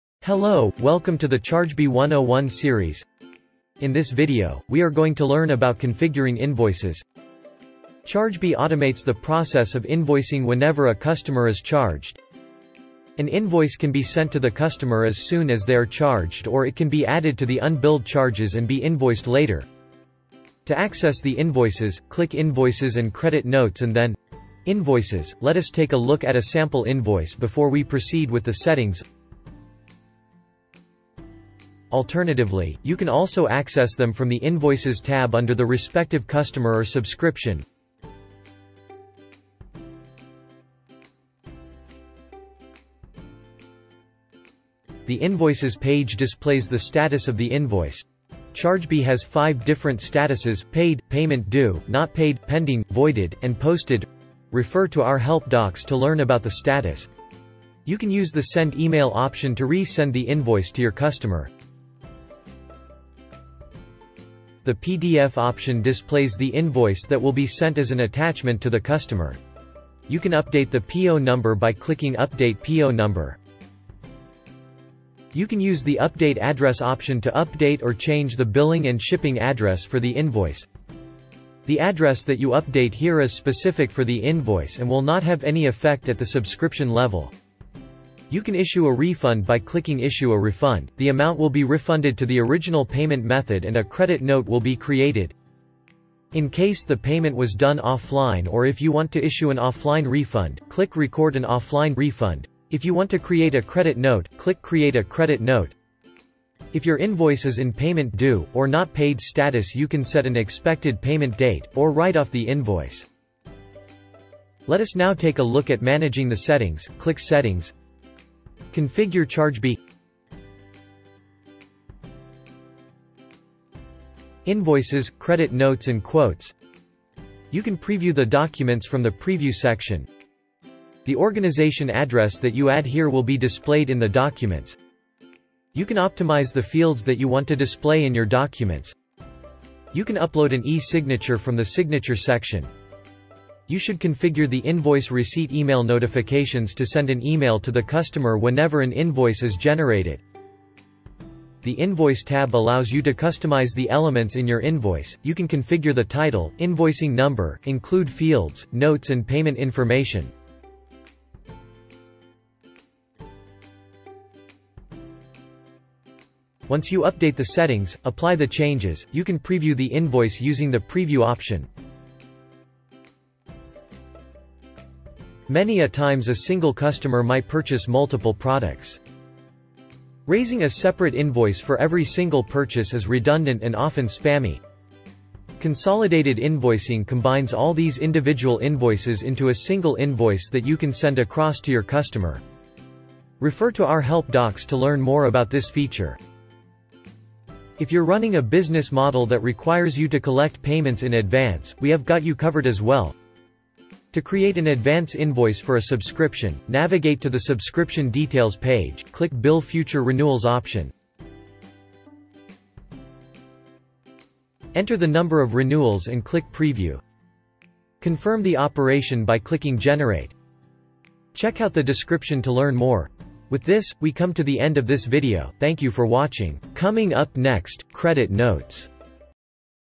Configuring Invoices
Chargebee automates the entire process of invoicing, starting from a new subscription sign up, when the invoice is created, to all payments made between the merchant and buyer. Email notifications can be sent out as well, informing buyers of the transaction status. Invoices | Credit Notes | Quotes